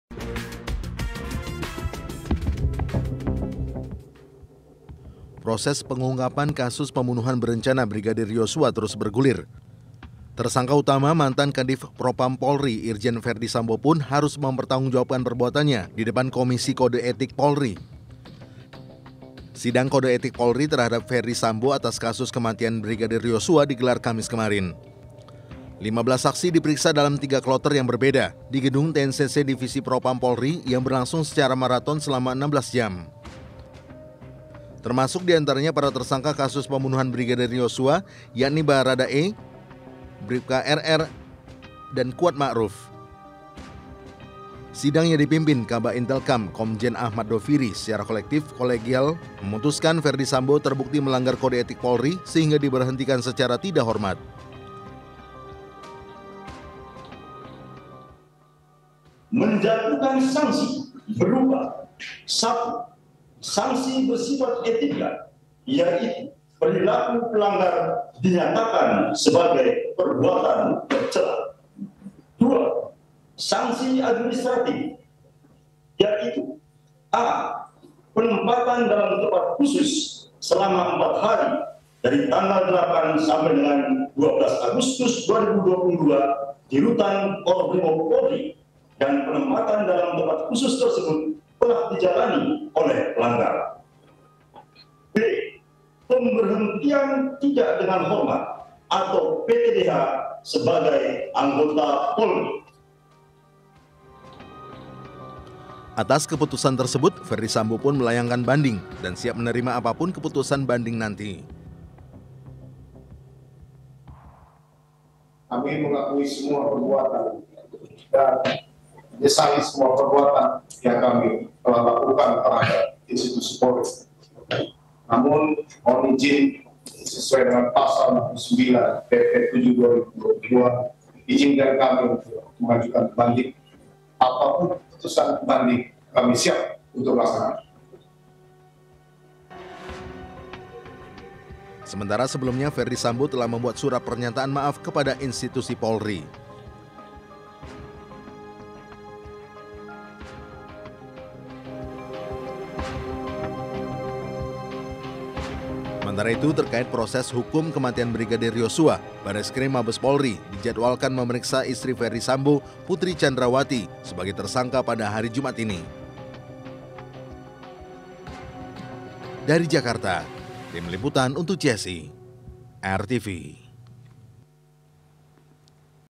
Irjen. Ferdy Sambo Dipecat dari Kepolisian
Kasus pembunuhan anggota polisi Brigadir J, akhinya mulai menemukan titik cerah. Di depan pimpinan Komite Kode Etik Polri, mantan Kadiv Propam Ferdy Sambo akui semua perbuatannya. Ferdy Sambo diputuskan diberhentikan tidak dengan hormat dari anggota Polri. Terkait hal tersebut, Ferdy Sambo mengajukan banding.